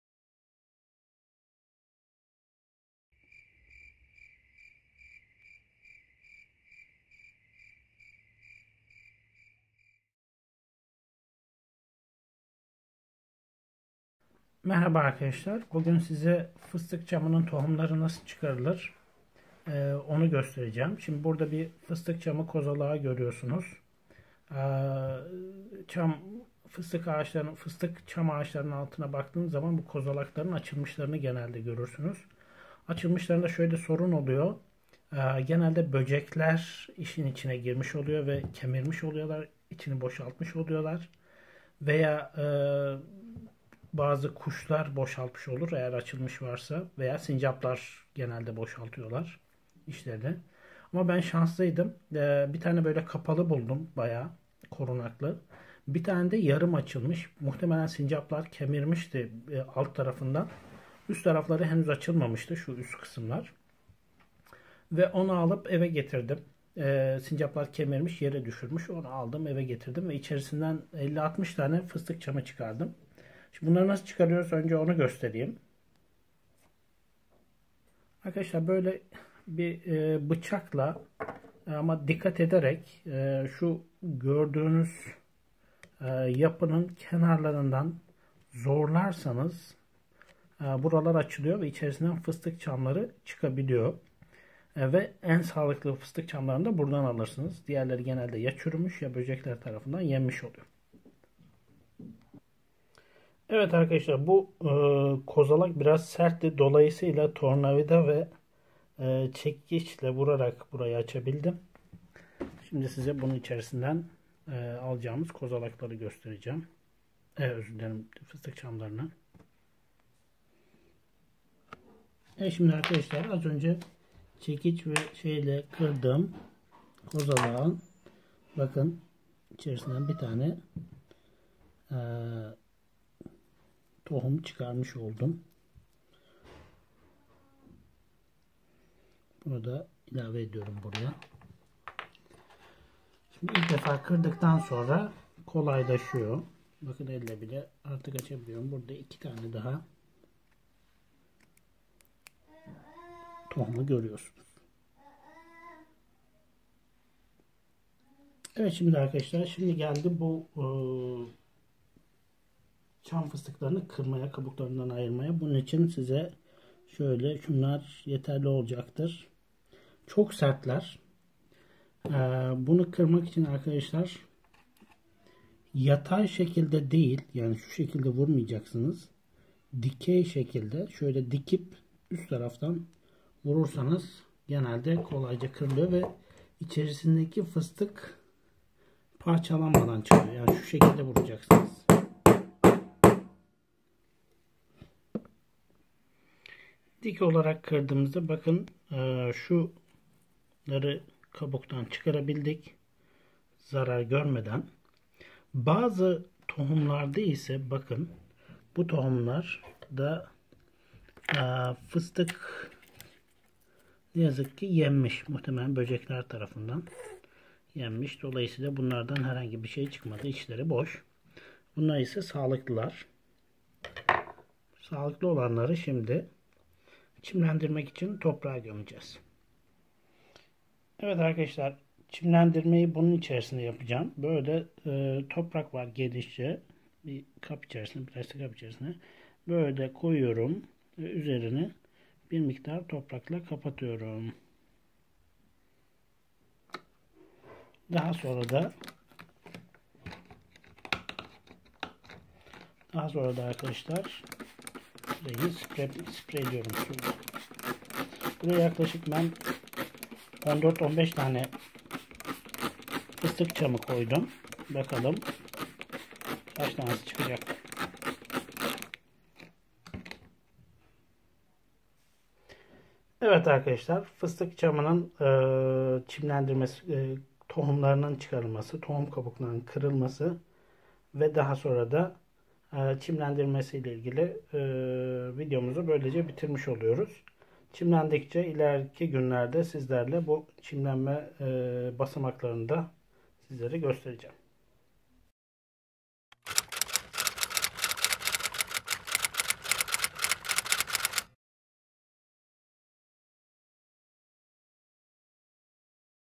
Collecting and germinating stone pine seeds / Fıstık çam tohumlarının toplanması ve çimlendirilmesi
EN: Video contains how to collect, break and germinate healthy stone pine seeds from the cones.
TR: Video sağlıklı fıstık çamı tohumlarının toplanmasını, kırılmasını ve çimlendirilmesini içermektedir.